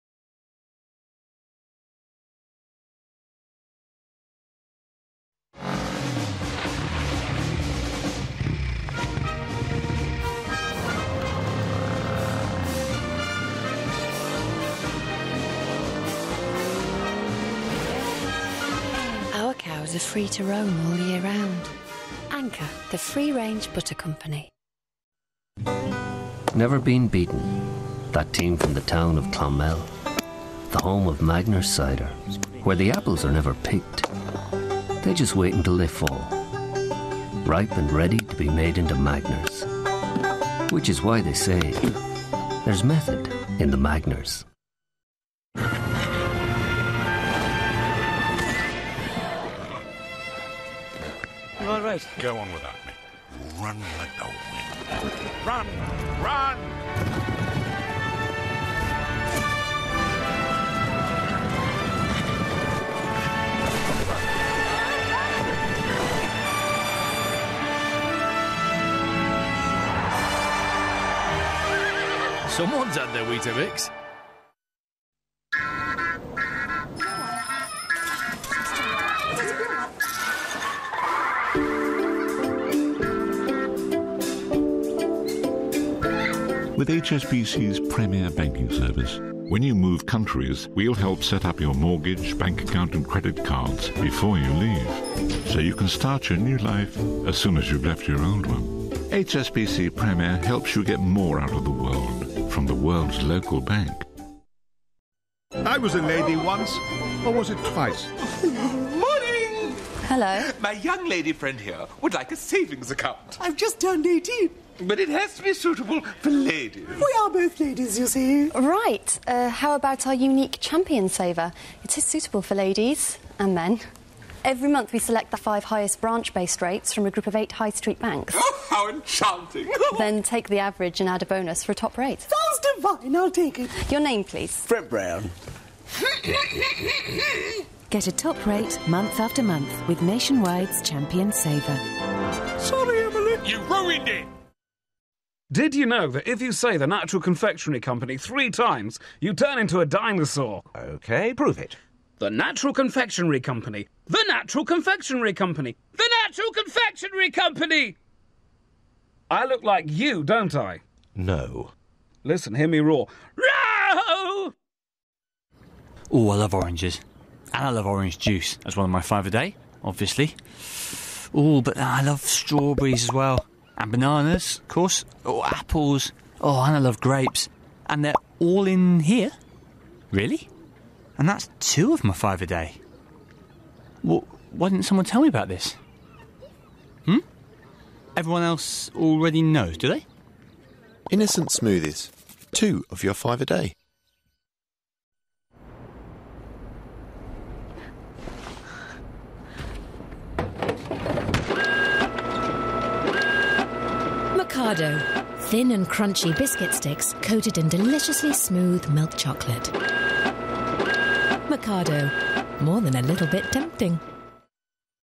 Adverts from the latter part of the 20th Century Volume 56
Anchor Butter (Great Escape Cow), Magners Cider (Cricket), Weetabix (Jockey), HSBC (Moving House), Nationwide (Ladies), The Natural Confectionery Company, Innocent Smoothies (Rabbit), Mikado (Photocopier).